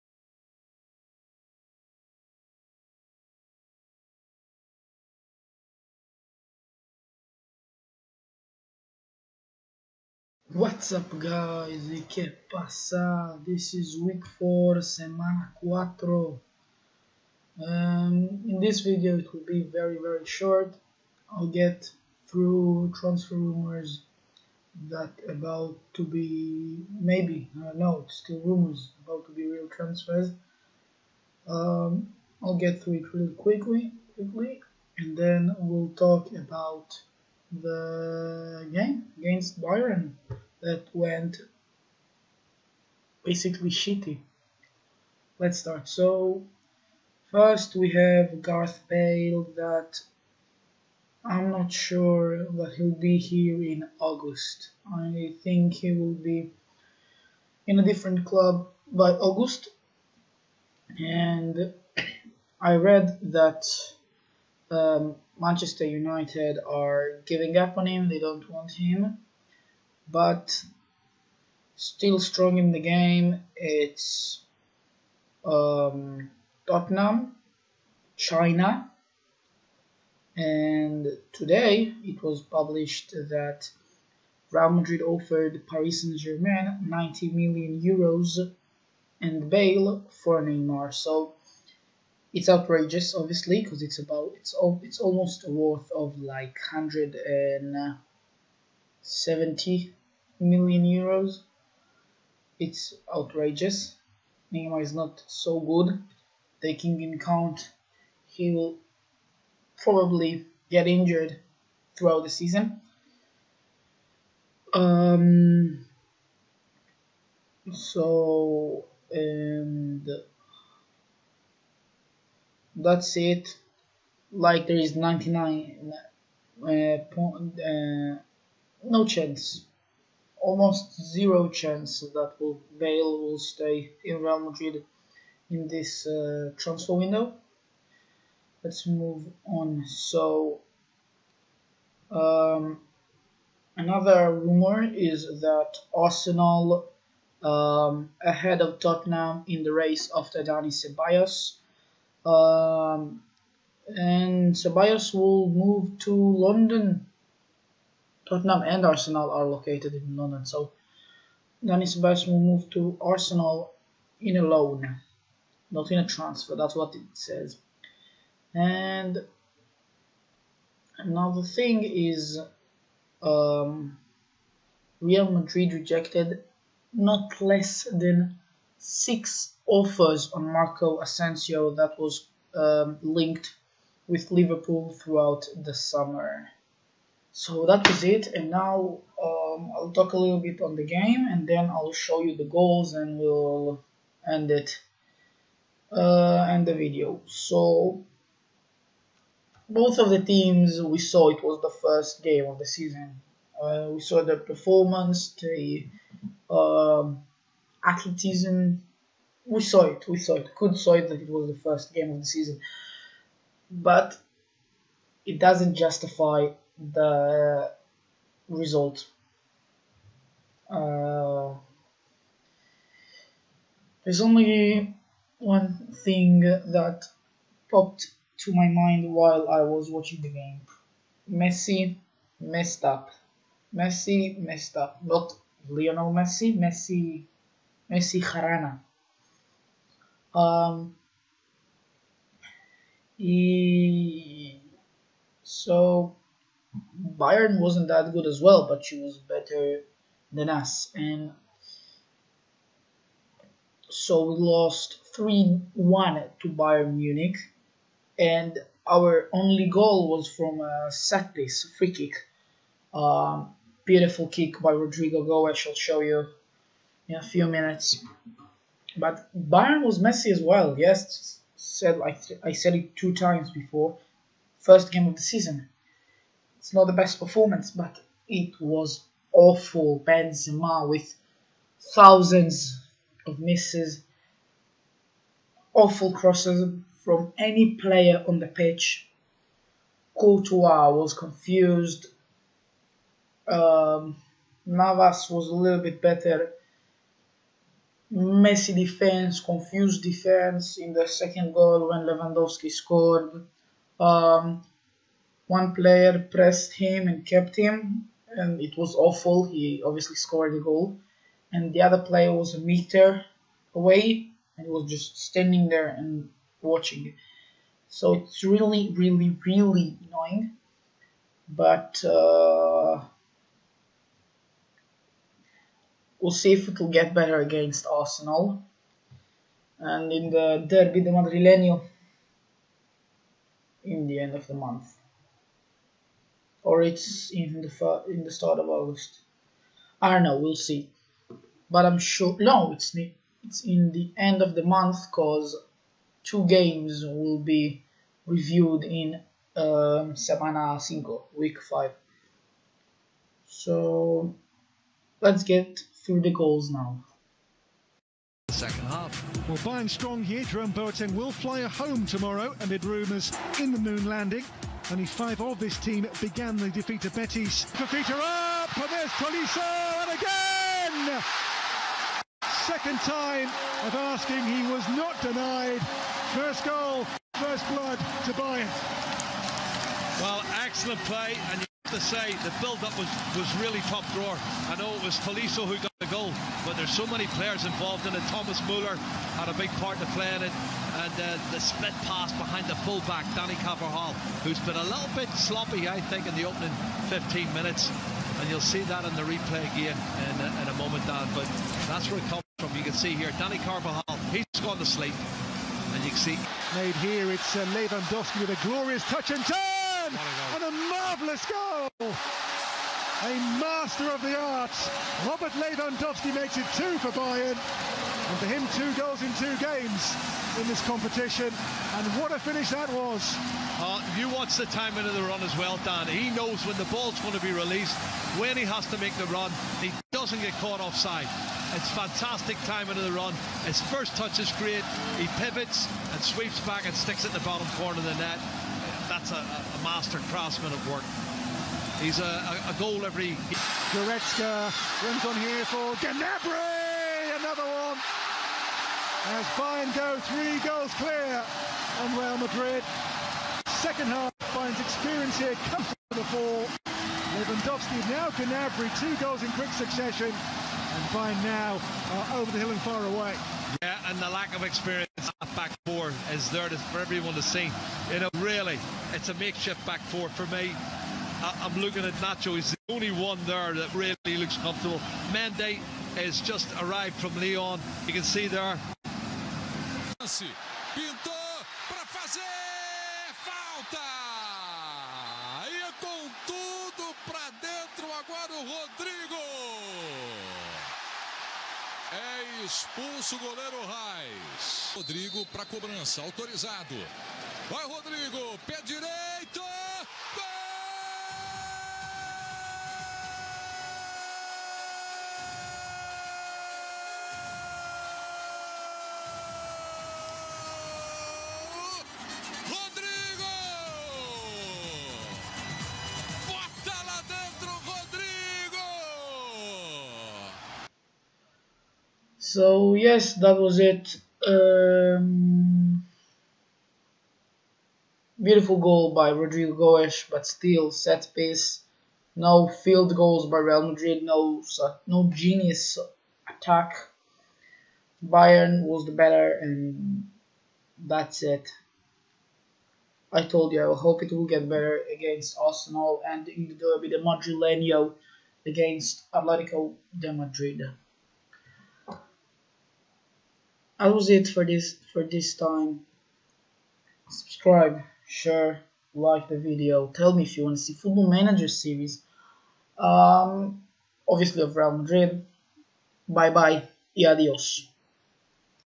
WEEK 4 | SEMANA 4
In this video, I'll talk about transfer rumors and Real Madrid's previous match versus Bayern Munich!